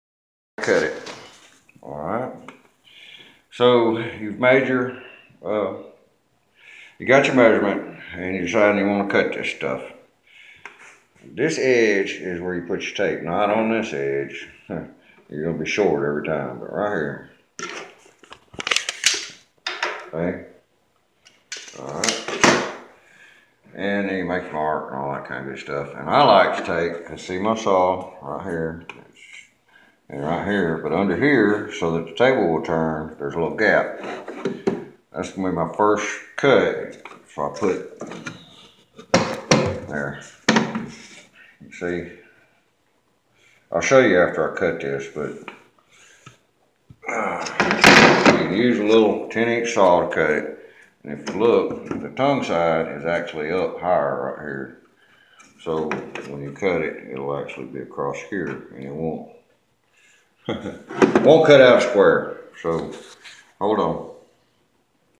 20180116 141326 cutting it square
note the direction your laying, with this stuff it's left to right not right to left . the grove faces you not the tongue. this is a tip on keeping square cuts against the walls
1st cut the grove is against the fence
Peace